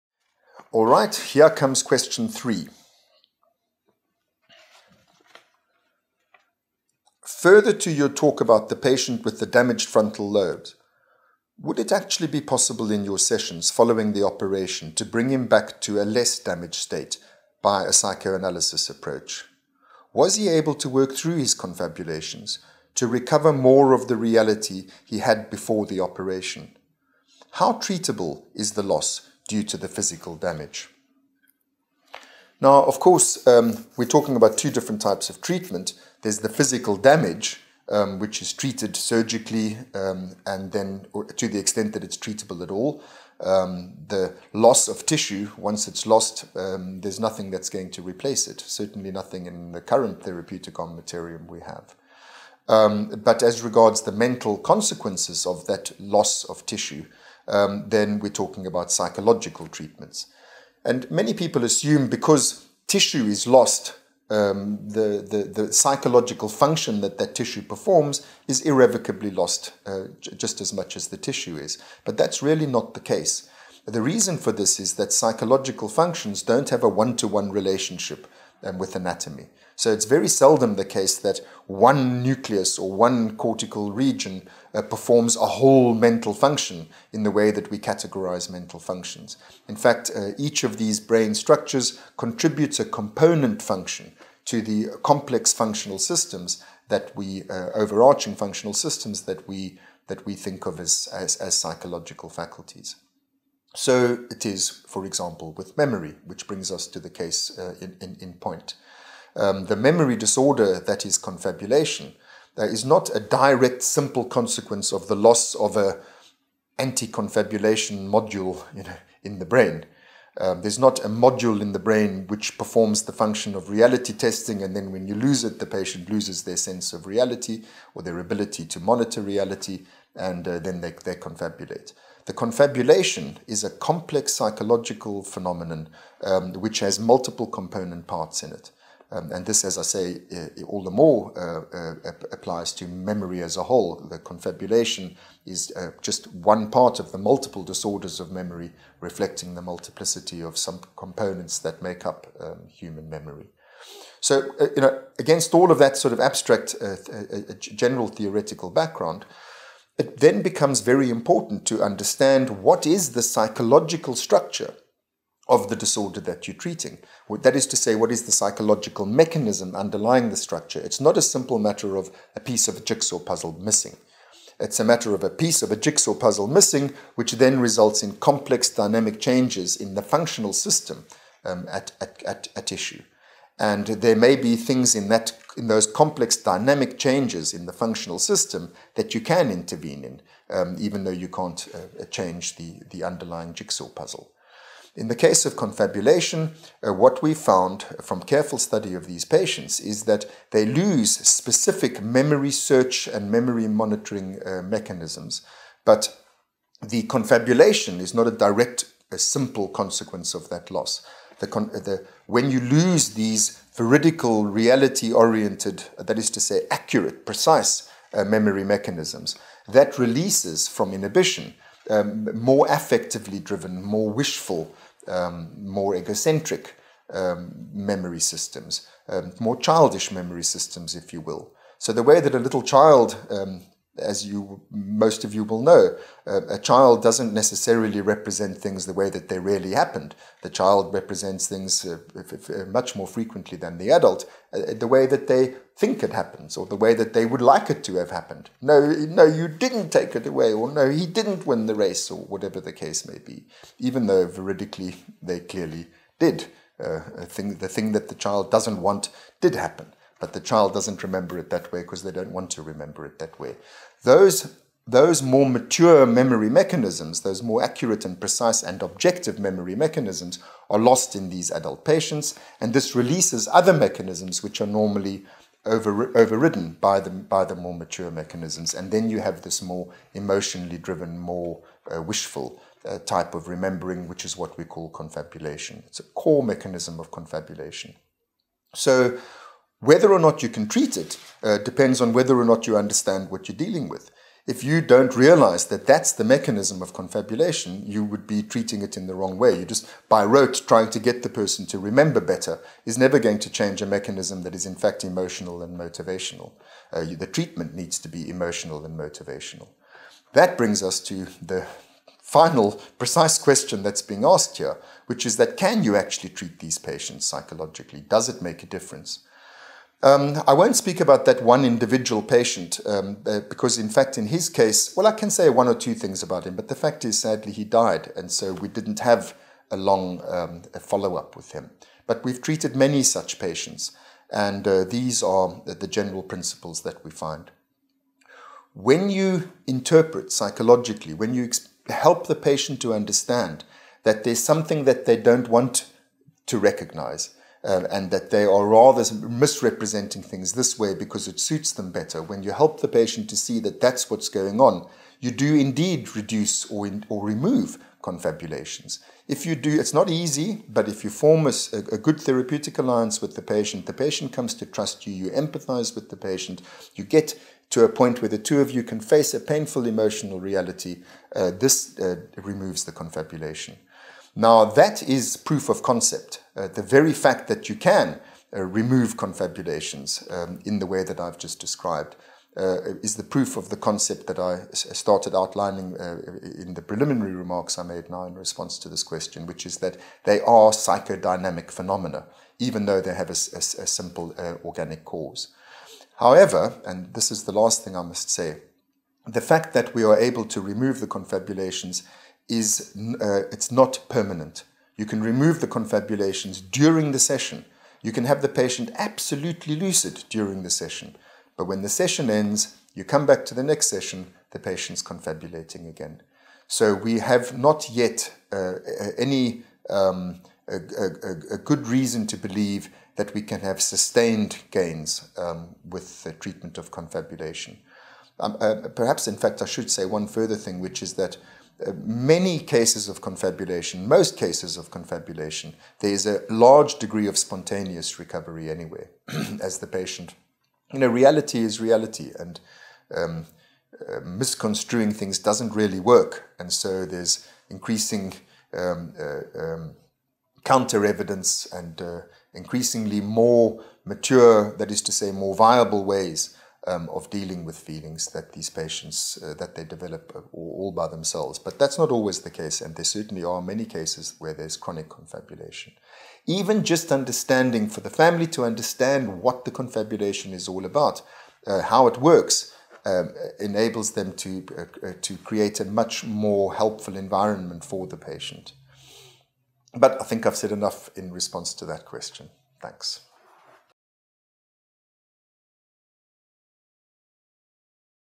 Ask Mark 6.7 - Treating physical damage with psychoanalysis?
Professor Mark Solms from the University of Cape Town (UCT) a question (see below) posed to him on UCT's free online course, "What is a Mind?", hosted by FutureLearn.

CC-BY-NC University of Cape Town. Visit creativecommons.org for more information on this licence.

Question: Further to your talk about the patient with the damaged frontal lobe: would it actually be possible in your sessions following the operation to bring him back to a less damaged state by the psychoanalysis approach? Was he able to work through his confabulations to recover more of the reality he had before the operation? How treatable is the loss due to the physical damage?